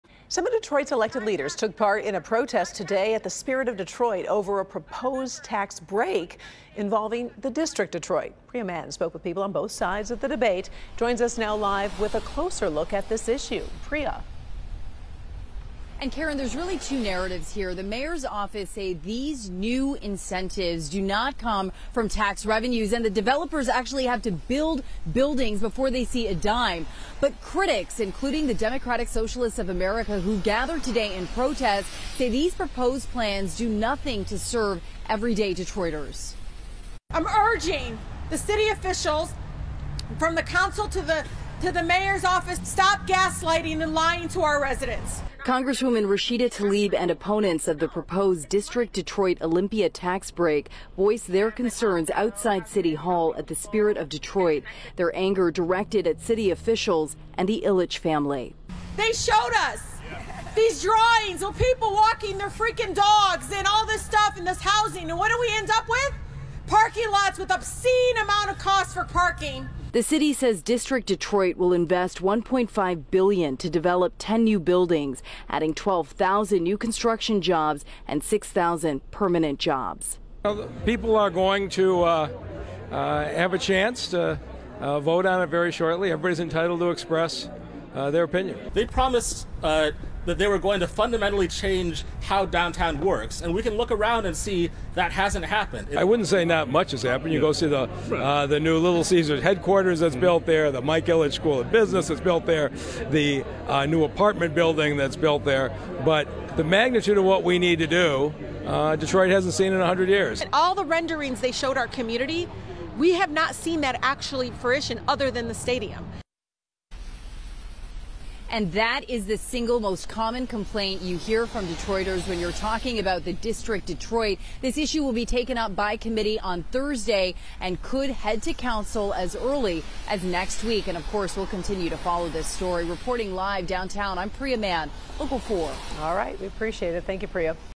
Group protests proposed Olympia tax break involving District Detroit
Some of Detroit’s elected leaders took part in a protest on Monday at the Spirit of Detroit over a proposed tax break involving the District Detroit.